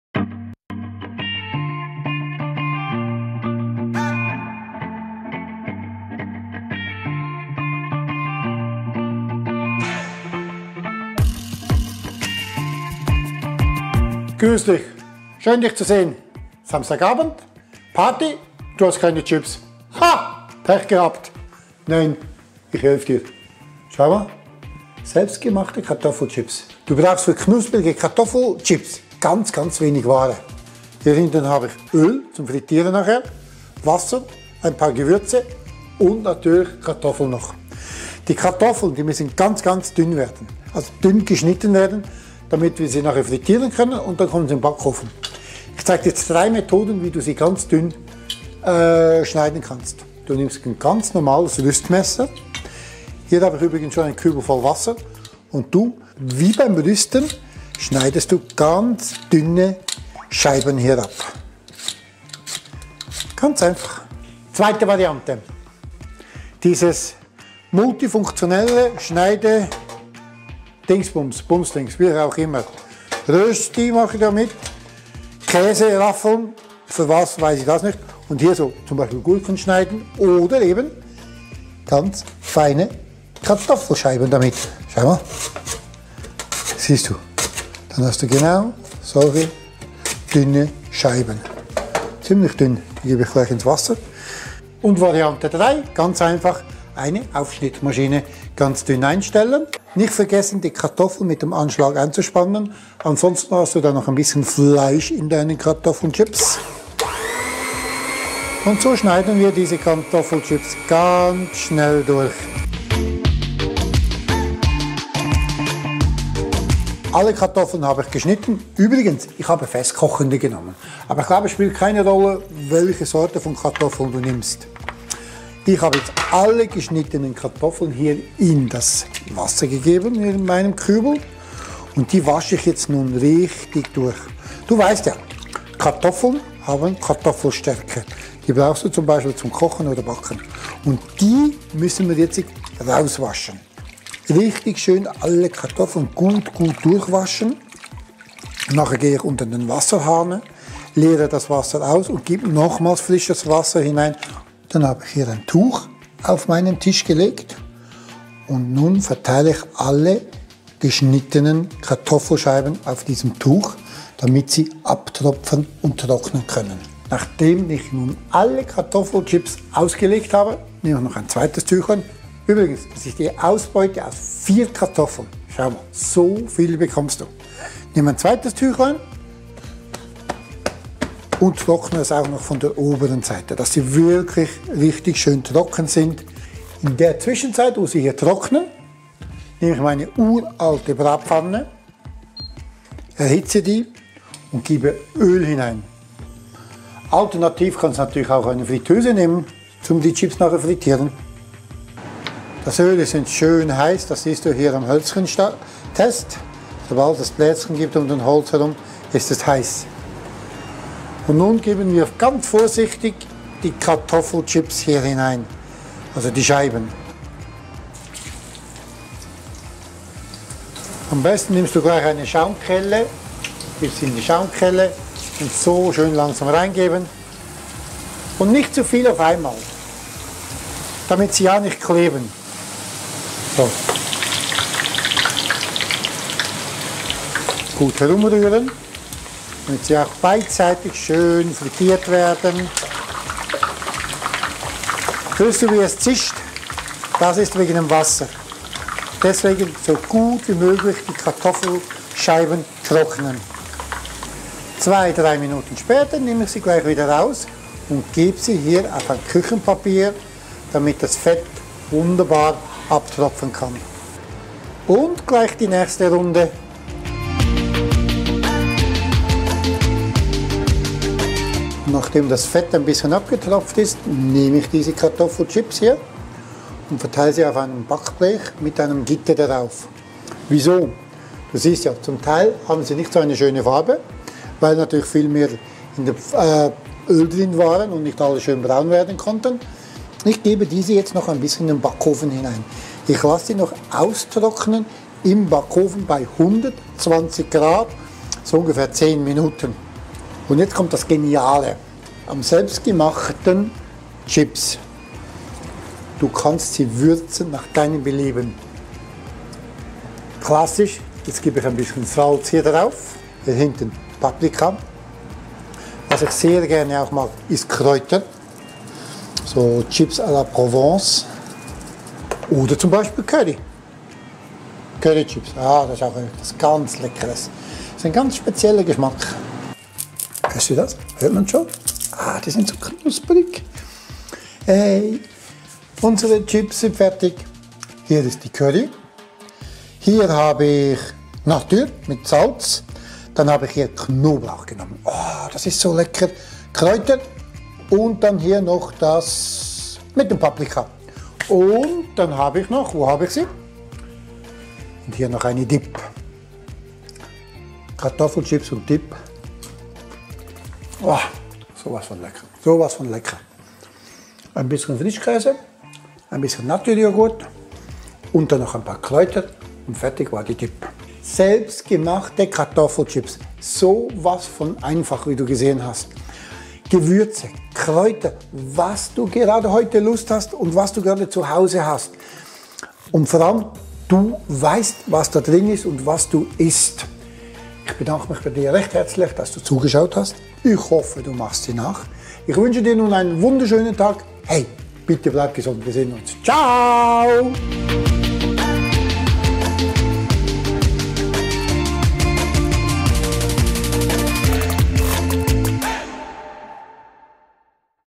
Kartoffelchips lecker und knusprig selber machen
Schon wieder keine Kartoffelchips zu Hause und du hast Lust darauf? Kein Problem. Ich zeige dir wie du im Handumdrehen leckere und knusprige Kartoffelchips machst.

Fragen, Kommentare sind immer herzlich willkommen.

--- Zeitstempel

0:00 Intro
0:13 Willkommen
0:26 Zutaten bereit stellen und Infos zum Schneiden
0:50 Dünne Scheiben schneiden Variante 1 - mit Rüstmesser
1:04 Dünne Scheiben schneiden Variante 2 - mit Raffel
1:34 Dünne Scheiben schneiden Variante 1 - mit Aufschnittmaschine
1:56 Kartoffelscheiben auswaschen
2:34 Kartoffelscheiben trocknen
3:07 Öl zum Frittieren bereit machen und erhitzen
3:30 Nun die Kartoffelscheiben frittieren und dann abtropfen
4:32 Die Chips auf ein Gitter legen zum austrocknen
5:00 Die Chips vor dem austrocknen im Backofen noch würzen
5:30 Die fertigen Chips mit einer Dipp anrichten und präsentieren
6:27 Danke fürs Zusehen und Tschüüüüssss
6:37 Zum Schluss nochmals den fertigen Kartoffelchips

--- Rezept zum Lesen

► Kartoffelchips

Kartoffel mit oder ohne Schale in ganz dünne Scheiben schneiden

wie denn oder mit was?
a mit dem Rüstmesser, das man sonst zum schälen braucht
b mit einer Raffel
c mit einer Aufschnittmaschine

Diese Scheiben dann im Wasser auswaschen, so dass
die Kartoffelstärke ausgewaschen wird.

Auf ein Tuch legen und das Wasser abtupfen/Abtrocknen

Gut abtrocknet, dann in heissem Öl (z. Bsp. Rapsöl) oder in einer Fritteuse frittieren.

Danach auf einem Küchentuch abtropfen.
Ich gebe sie dann nochmals für ein paar Minuten in den Backofen (ca 120 Grad),
damit sie richtig schön knusprig und braun werden.

Entweder nach dem trocknen im Backofen oder davor mit Gewürzen nach deiner Wahl würzen,
wie etwa Salz, Paprika, Knoblauch, Kräuter oder Curry usw.